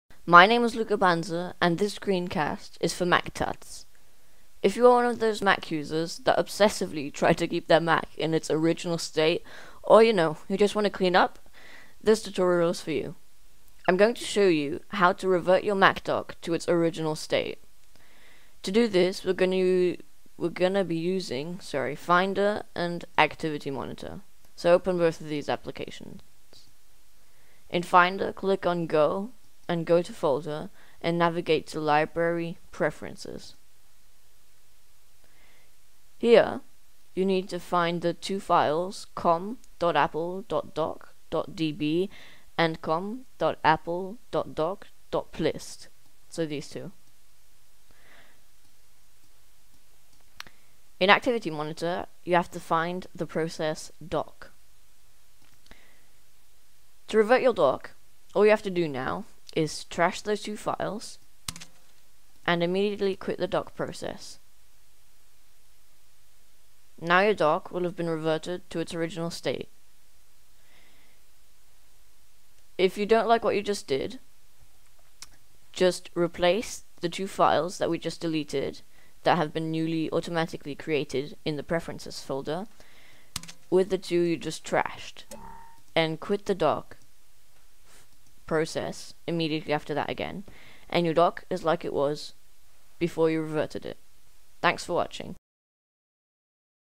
Revert Your Dock to its Original State
Learn how to reset your dock so it looks just like it did on the day you bought your Mac. Don't worry, you can undo this action!